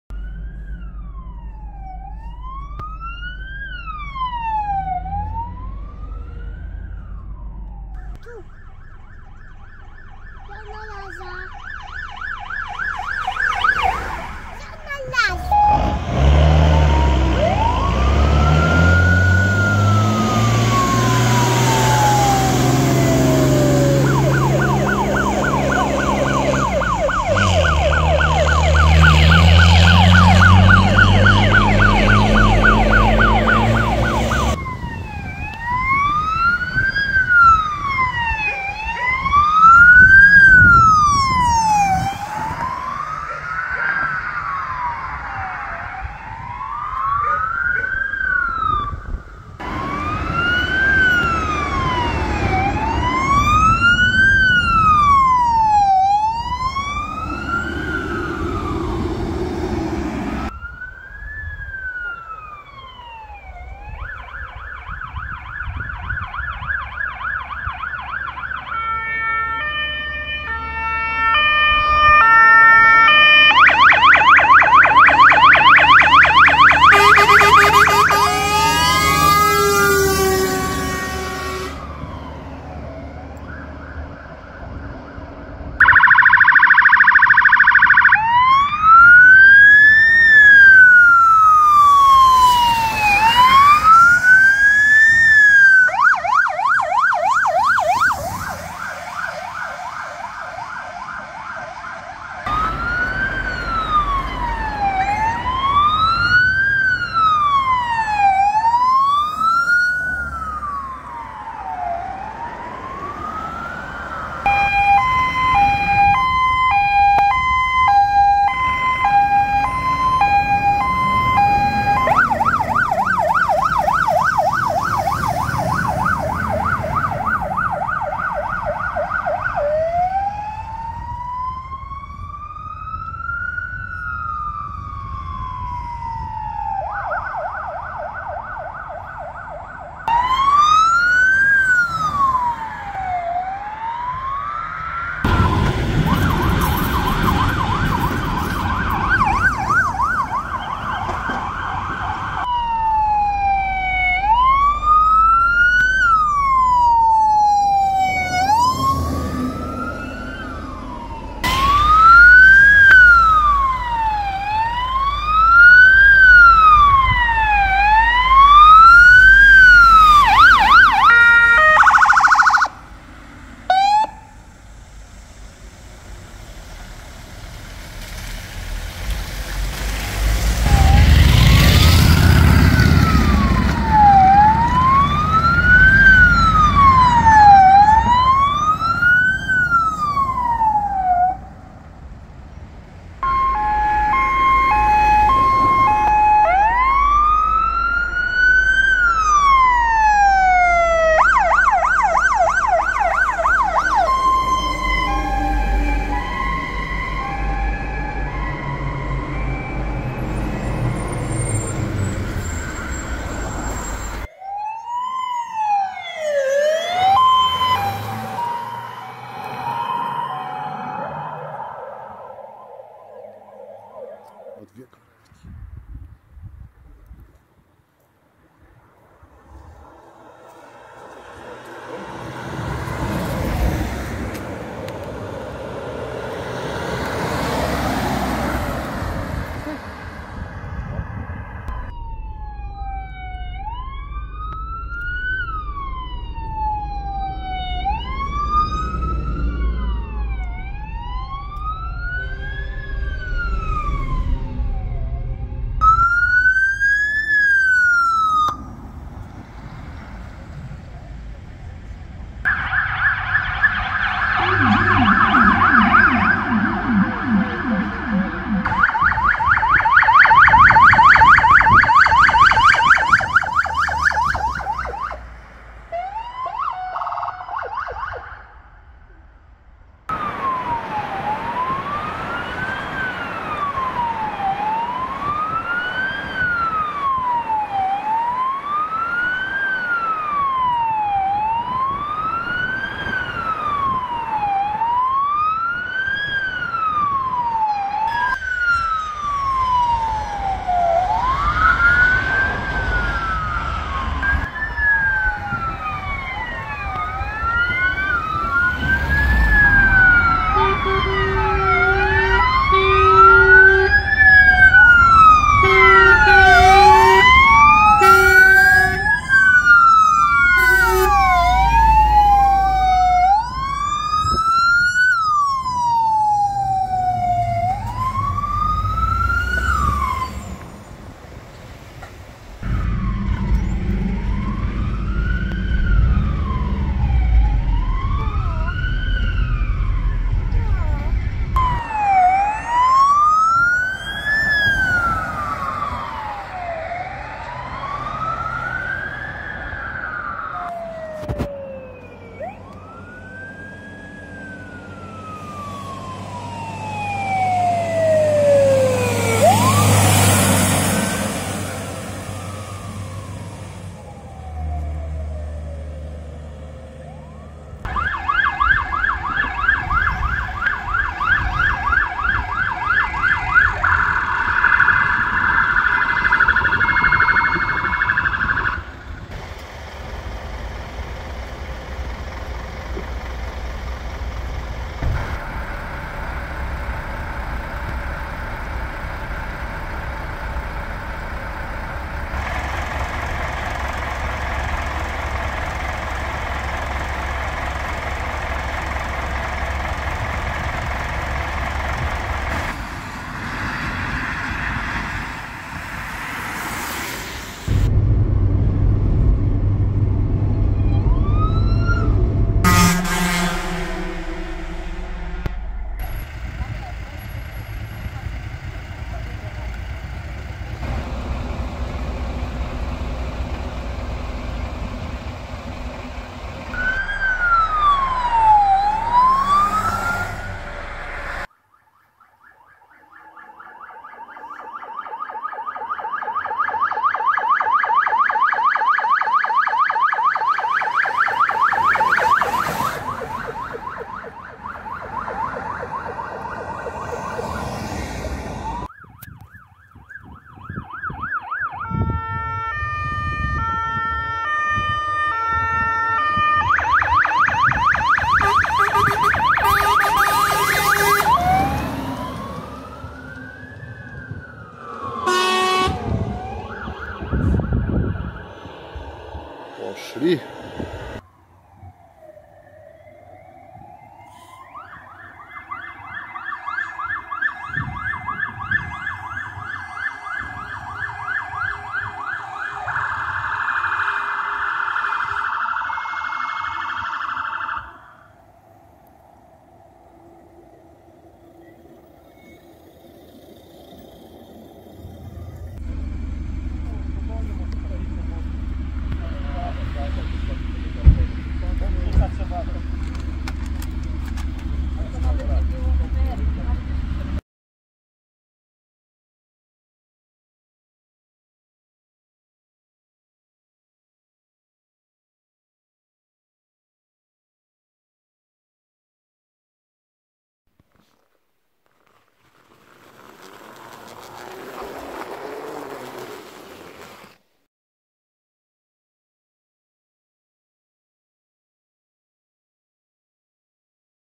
Alarmowo Policja Straż Pożarna Karetki Polska 2024r.
Witam, tu BatmanKłodzko, zapraszam na 9 minutową kompilacje z przejazdów alarmowych Policji , Straży Pożarnej oraz Karetek w Polsce. Wydanie 2024r. Zapraszam do oglądania.